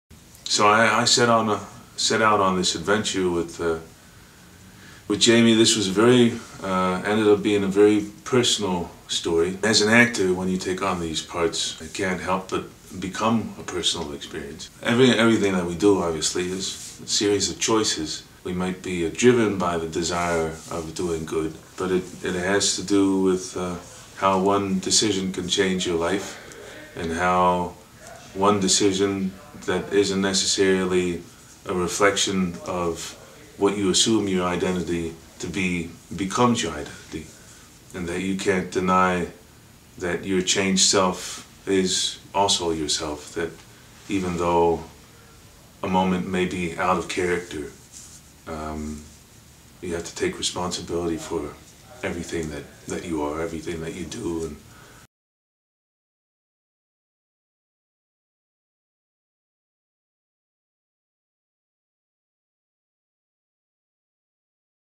Rossif Sutherland talks about telling personal stories in RIVER
Actor Rossif Sutherland talks to First Weekend Club about personal storytelling and shares his experiences filming RIVER.